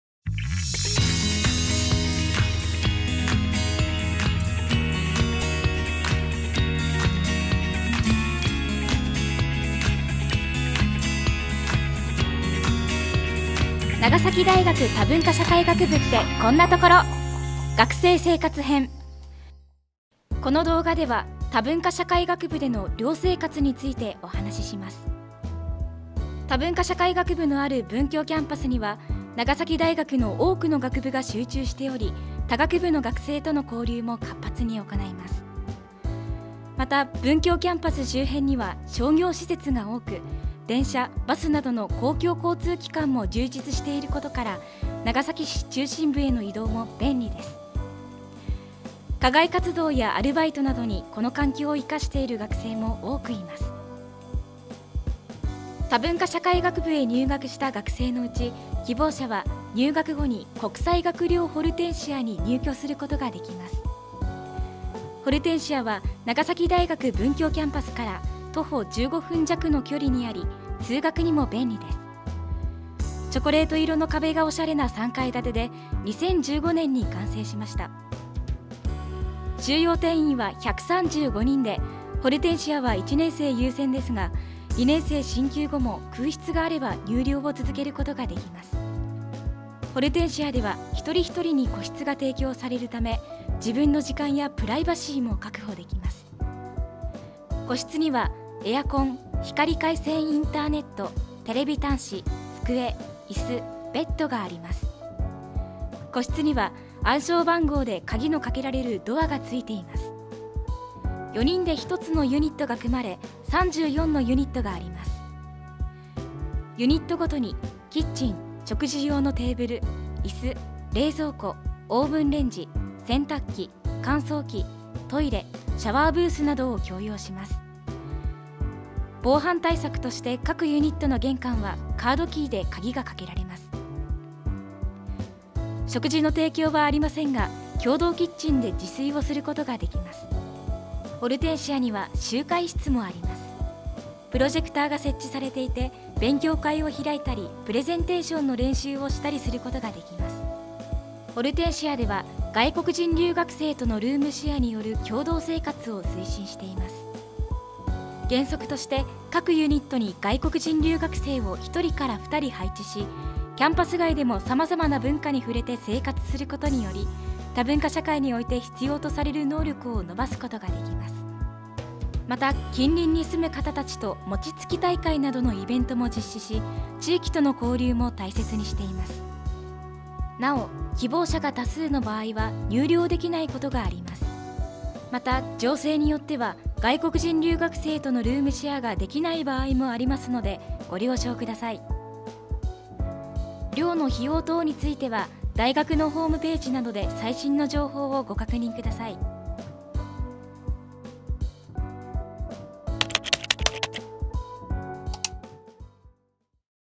長崎大学多文化社会学部ってこんなところ！～学生生活編～
長崎大学多文化社会学部の概要説明動画です。この動画では、主に学生交流に焦点を当てて説明します。